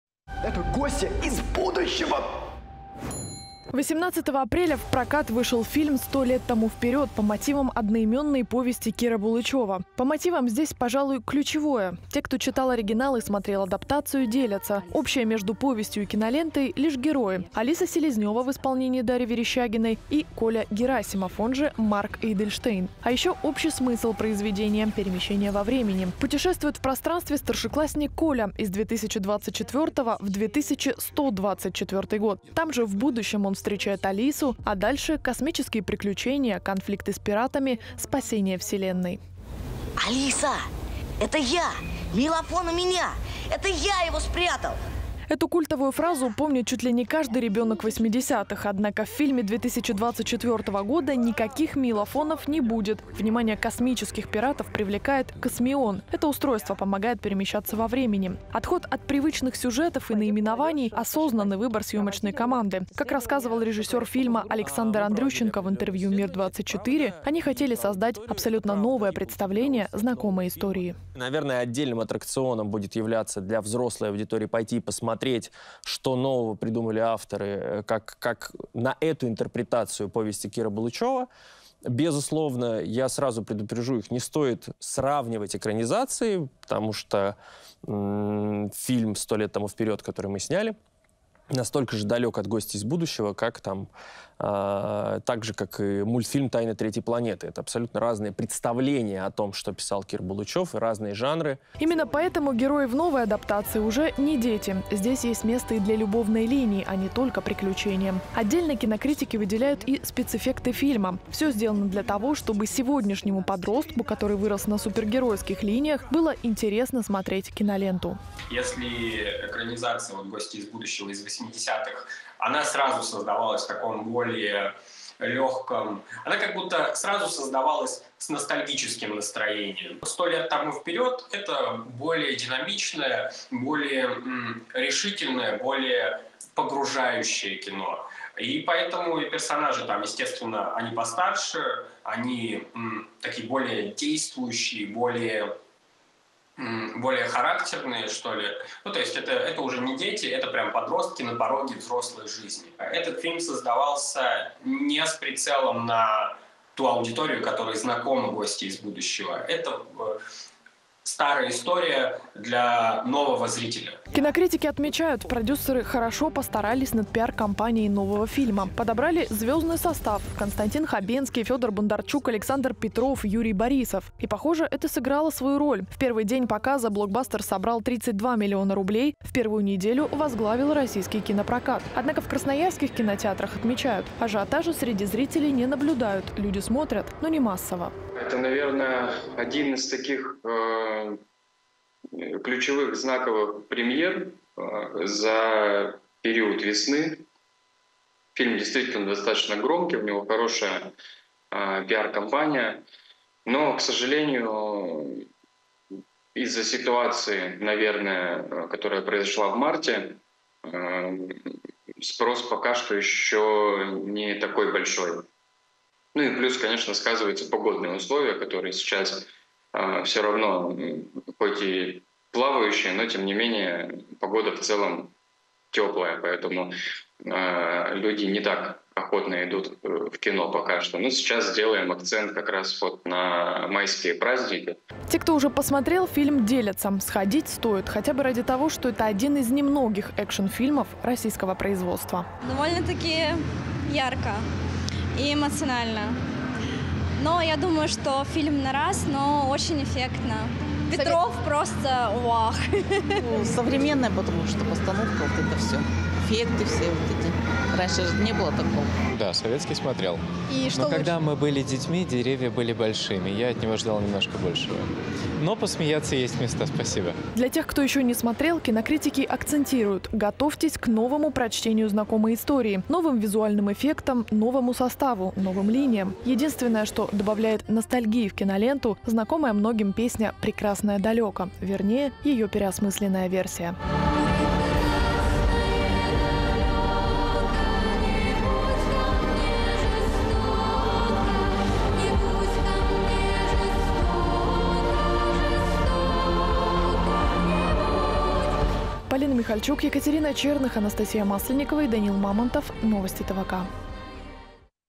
«Сто лет тому вперед» или «Гостья из будущего»? / мнение зрителей о новой киноадаптации
Новый фильм «Сто лет тому вперед» вторую неделю возглавляет российский прокат. Кинолента уже собрала больше 400 млн рублей. Люди охотно идут смотреть адаптацию повести Киры Булычева. В частности, чтобы сравнить с любимым многими советским фильмом «Гостья из будущего». Что изменилось за 40 лет после первой экранизации произведения? И есть ли потенциал у российской кинофантастики?